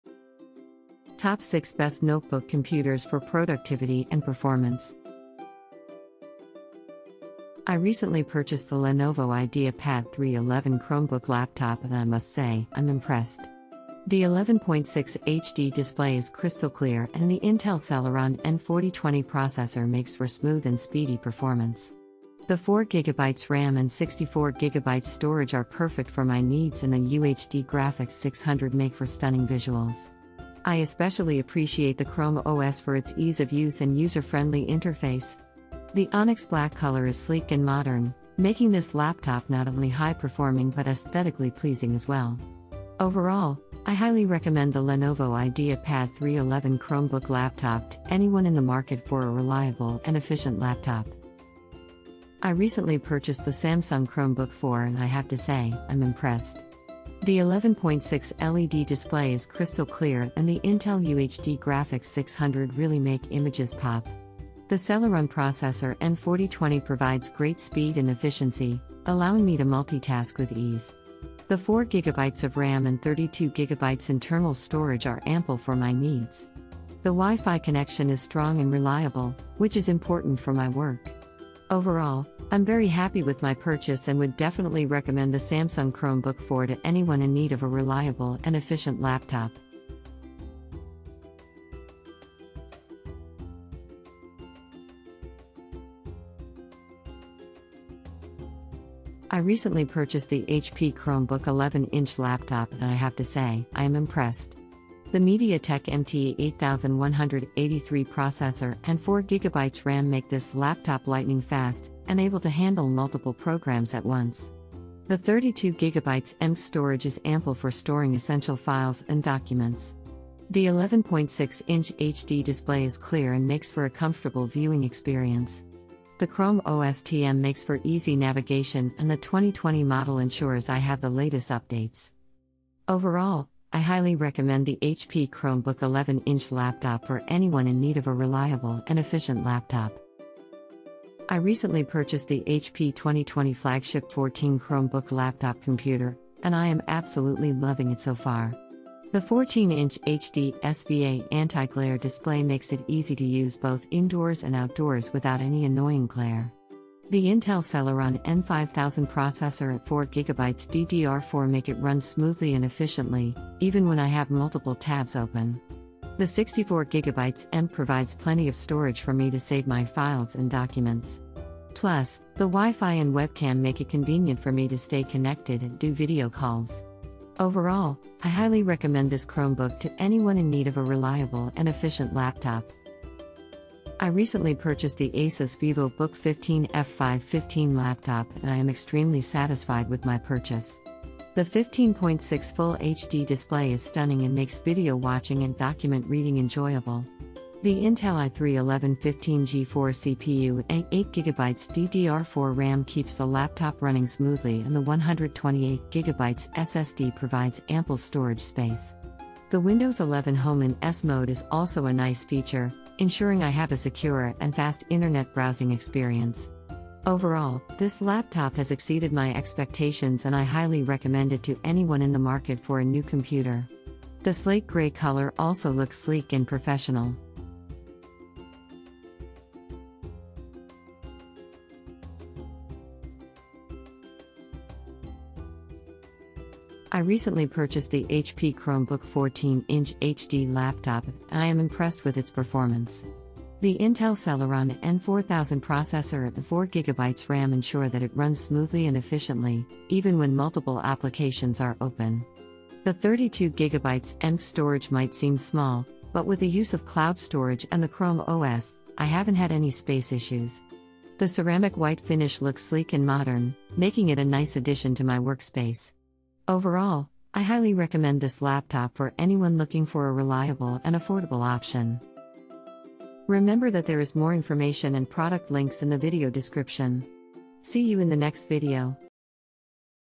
【NOTEBOOK COMPUTER】Top 6 Best Notebook Computers for Productivity and Performance
✋  Here are top 6 best notebook computers for productivity and performance available on the market.

🔽🔽🔽 PURCHASE LINKS NOTEBOOK COMPUTER 🔽🔽🔽

1) Lenovo IdeaPad 3 11 Chromebook Laptop,11.6" HD Display,Intel Celeron N4020, 4GB RAM, 64GB Storage, UHD Graphics 600, Chrome OS, Onyx Black

2) Samsung Chromebook 4 (2021 Model) 11.6" Intel UHD Graphics 600/ Celeron Processor N4020, 4GB, 32GB, Wi-Fi - (XE310XBA-KA1US)

3) HP Chromebook 11-inch Laptop - MediaTek - MT8183 - 4 GB RAM - 32 GB eMMC Storage - 11.6-inch HD Display - with Chrome OS™ - (11a-na0010nr, 2020 model)

4) HP 2020 Flagship 14 Chromebook Laptop Computer 14-inch HD SVA Anti-Glare Display Intel Celeron N5000 Processor 4GB DDR4 64GB eMMC WiFi Webcam Chrome OS (Renewed)

5) ASUS VivoBook 15 F515 Laptop, 15.6" FHD Display, Intel i3-1115G4 CPU, 8GB DDR4 RAM, 128GB SSD, Windows 11 Home in S Mode, Slate Grey, F515EA-AH34

6) HP Chromebook 14-inch HD Laptop, Intel Celeron N4000, 4 GB RAM, 32 GB eMMC, Chrome (14a-na0020nr, Ceramic White)

00:00 - Intro
00:06 - 1) Lenovo IdeaPad 3 11 Chromebook Laptop,11.6" HD Display,Intel...
00:51 - 2) Samsung Chromebook 4 (2021 Model) 11.6" Intel UHD...
01:38 - 3) HP Chromebook 11-inch Laptop - MediaTek - MT8183...
02:20 - 4) HP 2020 Flagship 14 Chromebook Laptop Computer 14-inch...
03:02 - 5) ASUS VivoBook 15 F515 Laptop, 15.6" FHD Display,...
03:55 - 6) HP Chromebook 14-inch HD Laptop, Intel Celeron N4000,...
04:32 - Outro